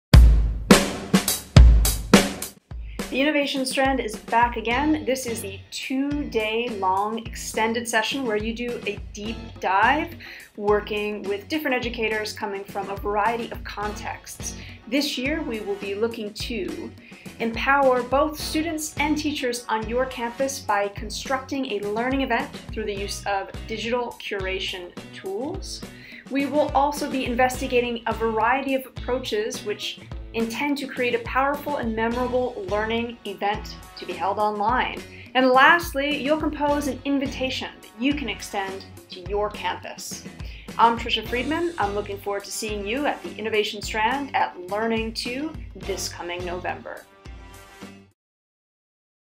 The Innovation Strand: Learning2 2018 Trailer
For years we've encouraged students to take ownership of their learning, to host their own online learning spaces, and to see creativity as a crucial component of their skills toolkit.  So what now?
Has your school helped students see online spaces as a place to spark action? Have you created learning experiences which authentically allow students to flex their fluency of ideas?

Curation with a Cause will provide time and provocations to help you dust off your digital curation tools for change. We will engage with curation as a means for facilitating action.  Come with a cause in mind, or come open-minded: this year's Innovation Strand is for those who see education as a change-making catalyst and are looking to construct an online event for their school community. In the Spirit of Blog Action Day, or John Spencer’s famous Maker Challenges, or The Podcast Challenge 2018 we will explore approaches to curate for a cause.